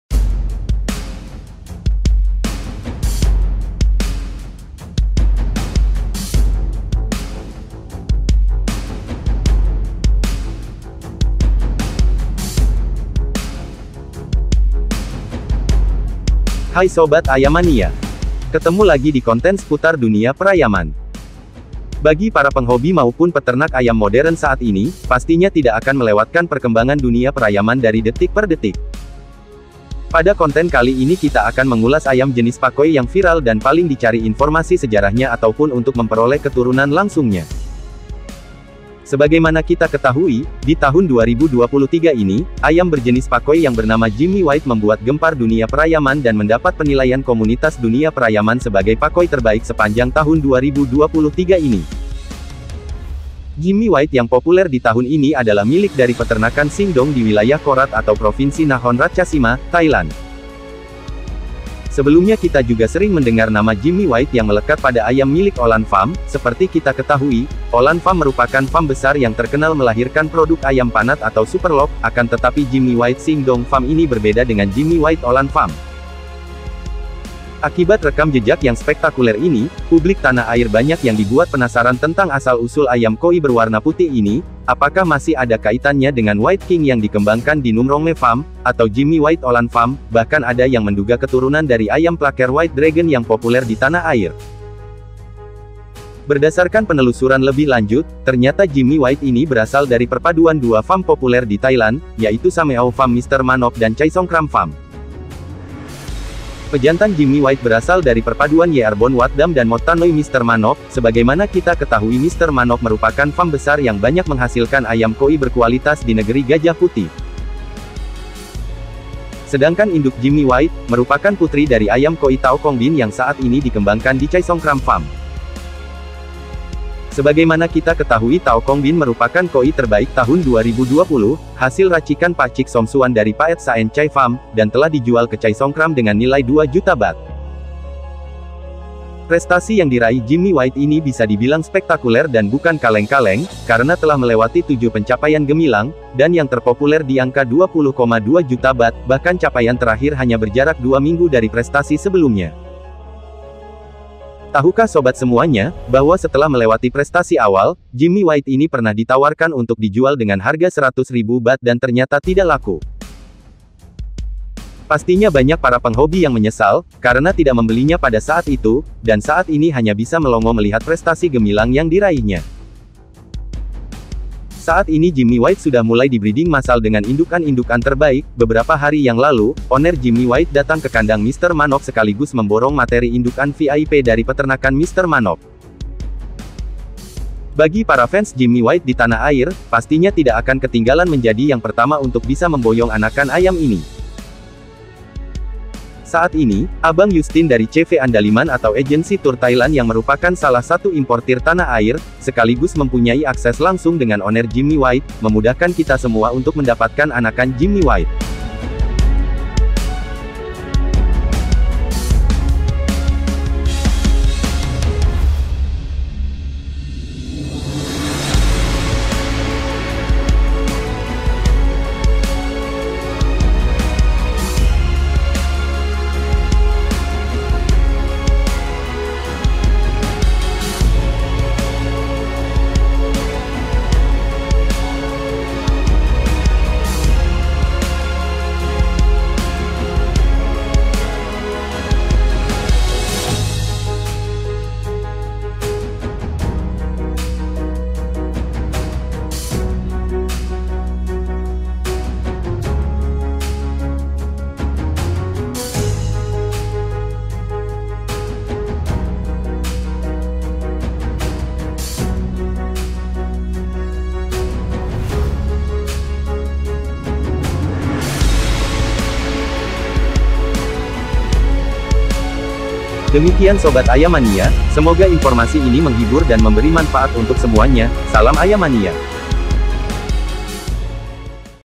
Sejarah Jimmy White, Ayam Pakhoy Terbaik Tahun 2023
Hai Sobat Ayamania...

Bagi para penghobi maupun peternak ayam modern saat ini, pastinya tidak akan melewatkan perkembangan dunia perayaman dari detik per detik. Sebagaimana kita ketahui, di tahun 2023 ini, ayam berjenis Pakhoy yang bernama Jimmy White  membuat gempar dunia perayaman dan mendapat penilaian komunitas dunia perayaman sebagai Pakhoy terbaik sepanjang tahun 2023 ini. Jimmy White  yang populer di tahun ini adalah milik dari Sing Dong Farm diwilayah Korat atau Provinsi Nakhon Ratchasima, Thailand.

Akibat rekam jejak yang spektakuler Pakhoy Jimmy White ini, publik banyak yang dibuat penasaran tentang asal usul ayam Khoy berwarna putih ini, apakah masih ada kaitannya dengan White King yang dikembangkan di Noomrongme Farm, atau Jimmy White Olan Farm, bahkan ada yang menduga keturunan dari ayam Plucker White Dragon yang populer ditanah air.

Berdasarkan penelusuran lebih lanjut, ternyata Jimmy White  ini berasal dari perpaduan dua farm populer di Thailand, yaitu Sameaw Farm Mr. Manop dan Chaisongkram farm. Pejantan Jimmy White berasal dari perpaduan Yearbon Watdam dan Modtanoi Mr. Manop. Sebagaimana kita ketahui Mr. Manop merupakan farm besar yang banyak menghasilkan ayam khoy berkualitas di negeri gajah putih.

Sedangkan induk Jimmy White merupakan putri dari ayam Khoy Taokongbin yang saat ini dikembangkan di Chaisongkram Farm. Sebagaimana kita ketahui Taokongbin merupakan khoy terbaik tahun 2020 hasil racikan Pakcik Somsuan dari Paed Saen Chai Farm dan telah dijual ke Chaisongkram dengan nilai 2 juta bath.

Tahukah sobat semuanya, Jimmy White  ini pernah ditawarkan untuk dijual dengan harga 100 ribu bath dan ternyata tidak laku. Pastinya banyak para penghobi yang menyesal, karena tidak membelinya pada saat itu, dan saat ini hanya bisa melongo melihat prestasi gemilang yang diraihnya.

Saat ini Jimmy White  sudah mulai dibreeding masal dengan indukan terbaik. beberapa hari yang lalu Owner Jimmy White  datang kekandang Mr. Manop sekaligus memborong materi indukan V I P dari peternakan Mr. Manop. Nah.. Pastinya sobat semuanya sangat tertarik untuk memboyong anakan Jimmy White ini.